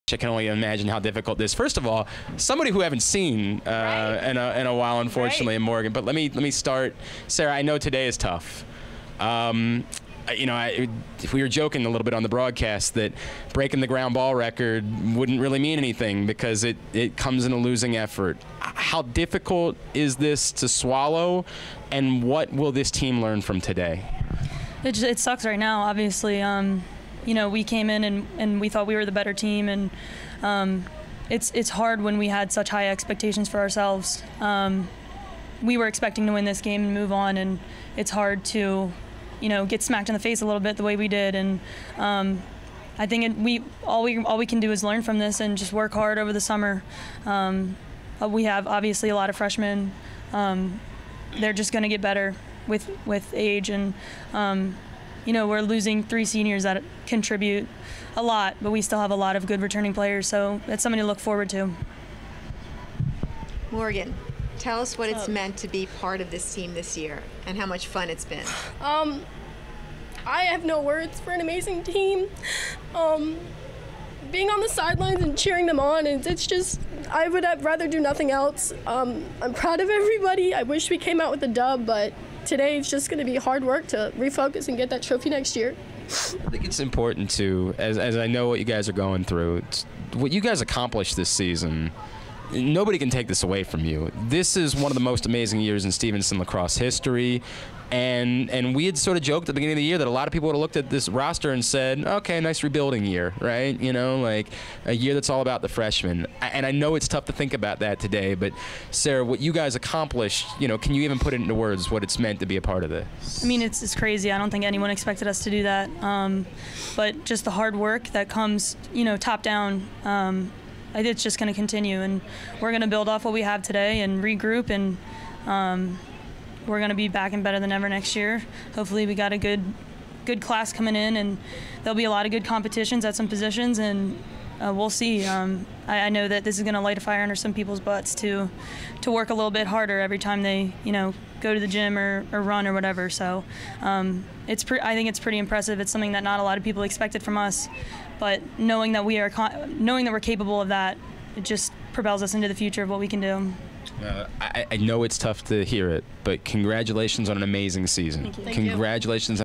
Women's Lacrosse MAC Championship Postgame with Morgan Bateman and Sarah Roerty
Sarah Roerty and Morgan Bateman talk about the team's game with Lebanon Valley this afternoon.